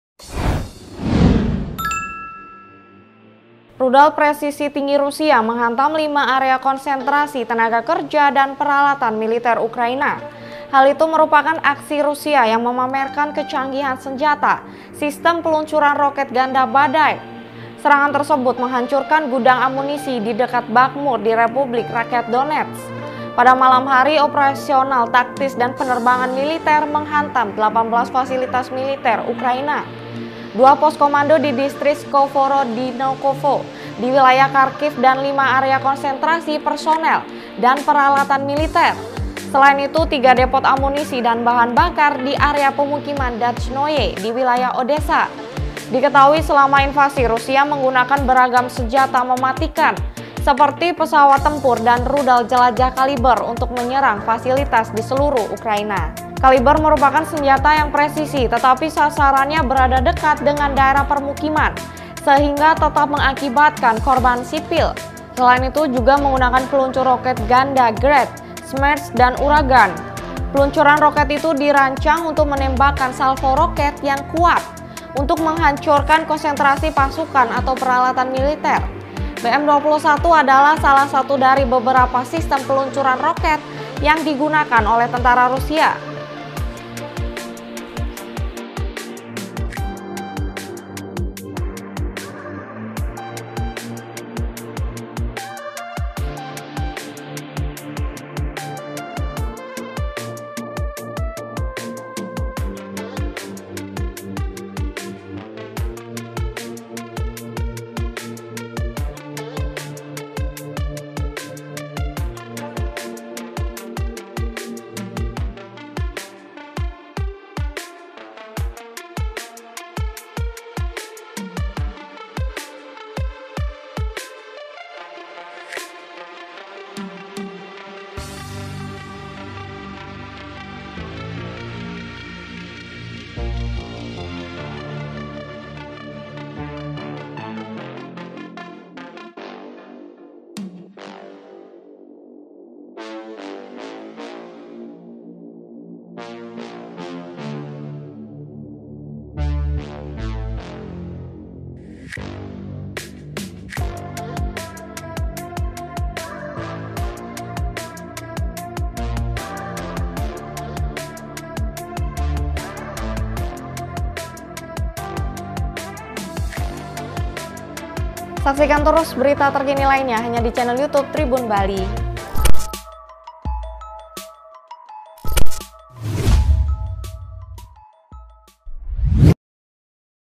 Video Militer Rusia Pamer Kecanggihan Roket Ganda Badai, Bisa Hancurkan Tentara Ukraina dari Jauh
Rudal presisi tinggi Rusia menghantam lima area konsentrasi tenaga kerja dan peralatan militer Ukraina.

Hal itu merupakan aksi Rusia yang memamerkan kecanggihan senjata sistem peluncuran roket ganda Badai.

Serangan tersebut menghancurkan gudang amunisi di dekat Bakhmur di Republik Rakyat Donetsk.

(paw)